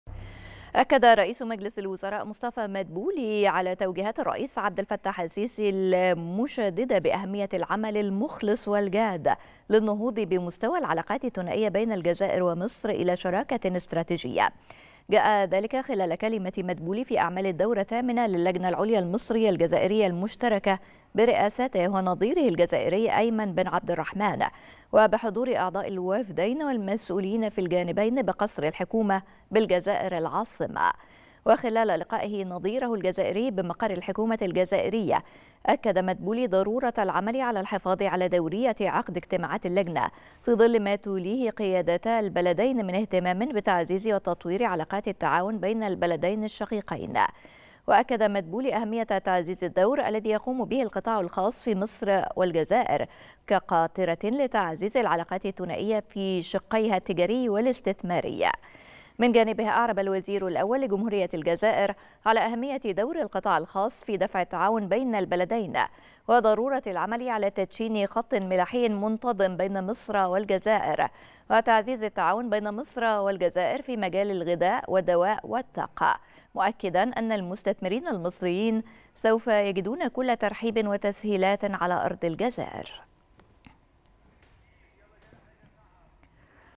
مدبولي: توجيهات من الرئيس السيسي بأهمية العمل الجاد للنهوض بمستوى العلاقات إلى شراكة استراتيجية
نشرة التاسعة | مدبولي: توجيهات من الرئيس السيسي بأهمية العمل المخلص والجاد للنهوض بمستوى العلاقات إلى شراكة استراتيجية